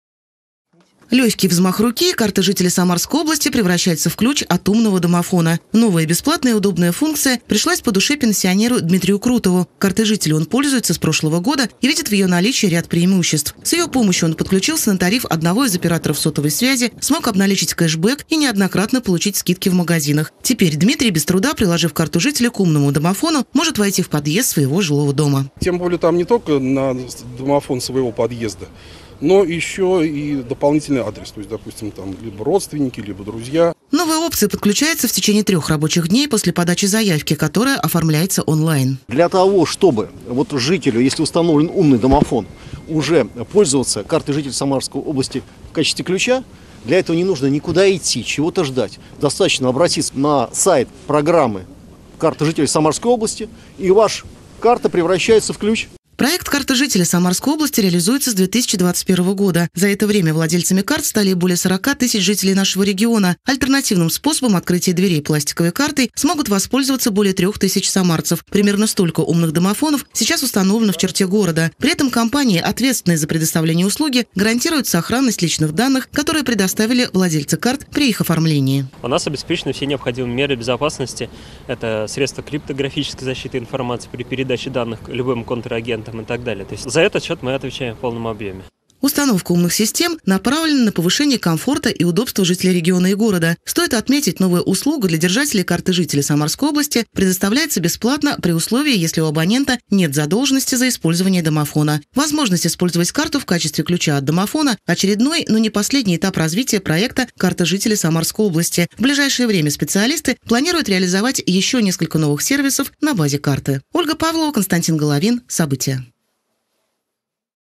У Карты жителя Самарской области появилась новая полезная опция
Умная система для повышения комфорта и удобства жителей региона и города. При помощи карты жителя Самарской области теперь можно не только обналичить кэшбек, приобрести билеты на проезд в общественном транспорте, получить скидки, но и войти в подъезд своего жилого дома, приложив пластиковую карту к домофону. Как оформить услугу и сколько ждать синхронизации карты с умным домофоном, - узнала Ольга Павлова.